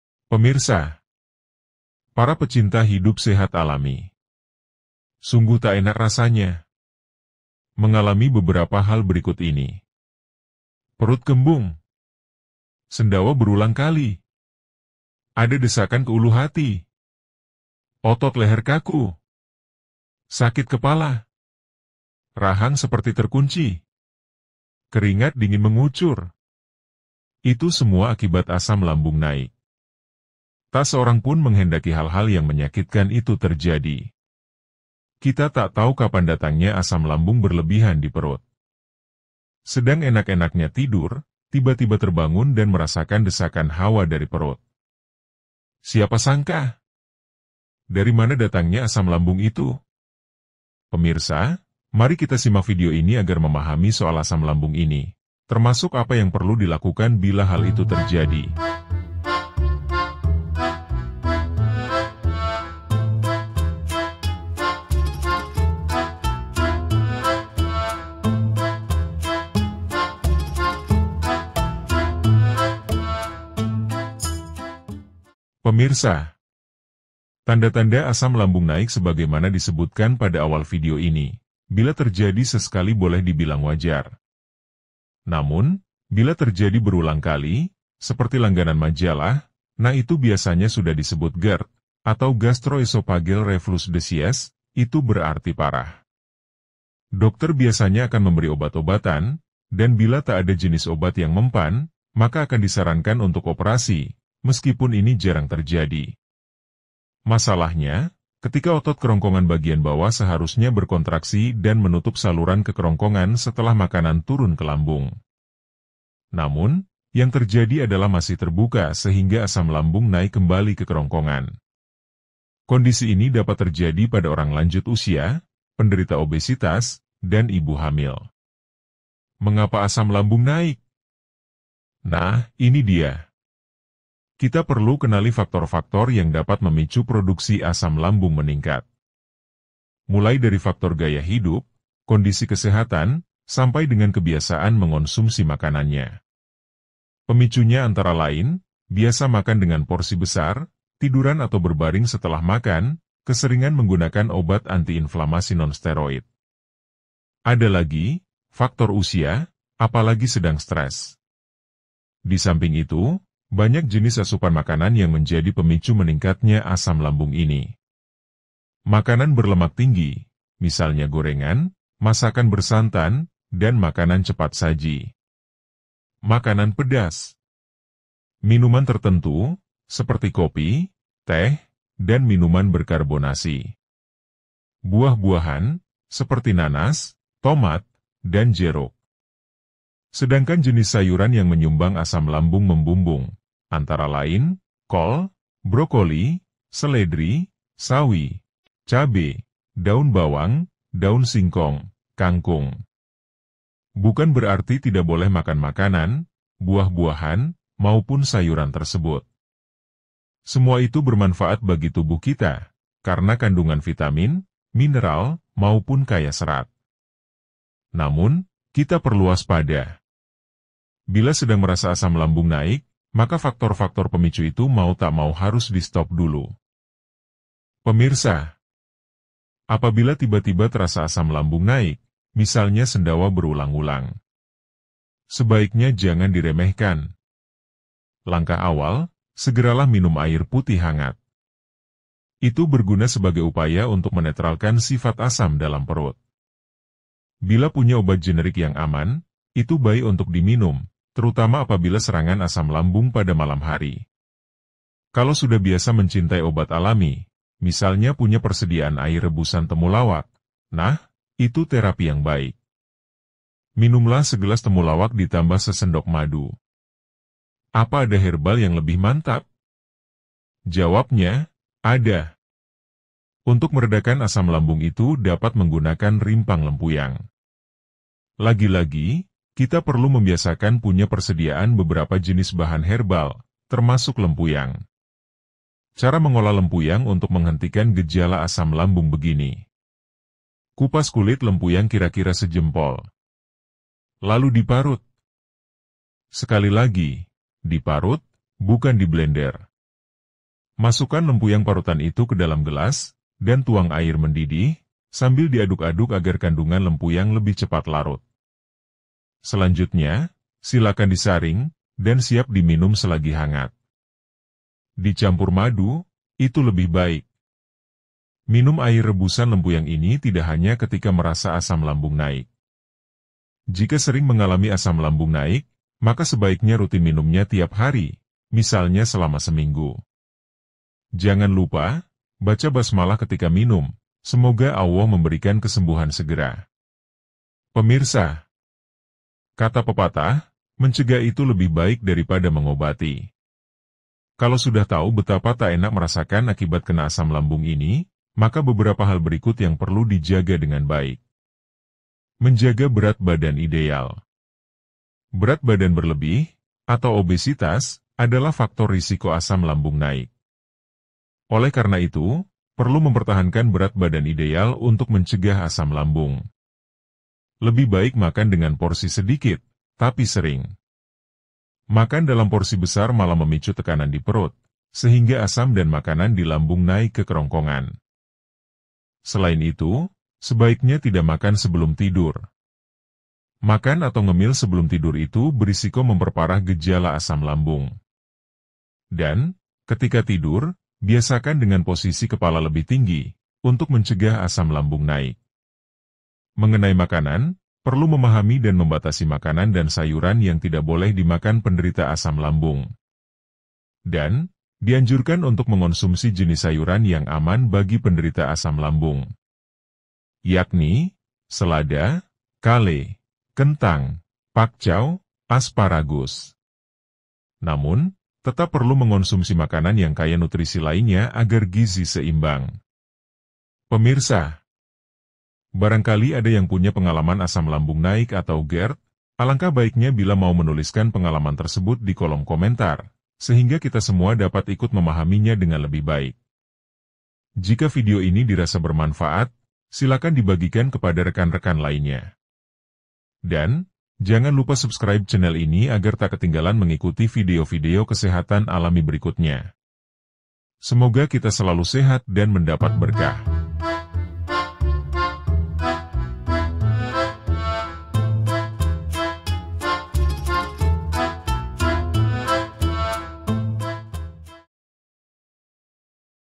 Lempuyang efektif untuk asam lambung dengan herbal
Bila asam lambung terjadi berulang kali, seperti langganan majalah, nah itu biasanya sudah disebut GERD, atau gastro-esophagel reflux decies, itu berarti parah . Sebaiknya jangan remehkan bila sudah tahu tanda-tanda asam lambung naik. Ada cara alami, menggunakan herbal, pengalaman menunjukkan efektif meredakannya. Perlu rutin mengonsumsi lempuyang, dengan cara dijelaskan secara detail dalam video ini. Silakan simak sampai tuntas supaya dapat memahaminya dengan baik. Semoga kita selalu sehat wal afiat.

Cara menghela asam lambung dengan herbal